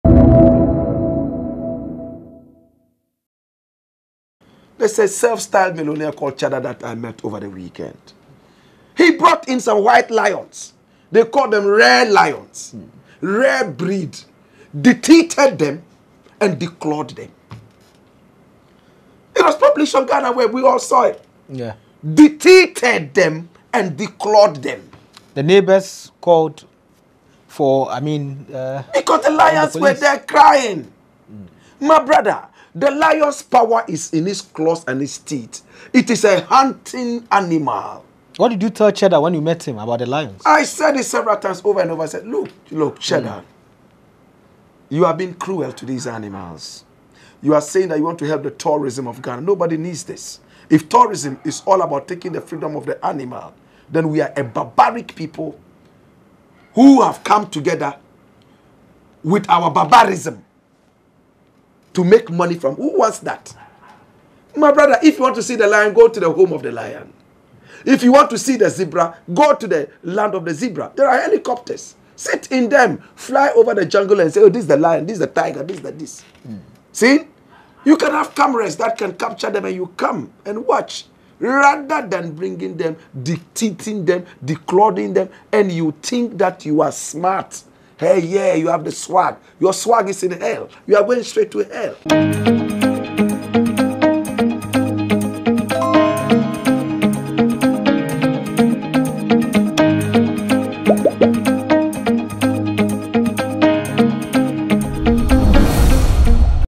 I told Cheddar he’s being cruel for deteething, declawing lions - Blakk Rasta
Blakk Rasta addresses anima rights and why Cheddar is cruel for deteething, declawing lions.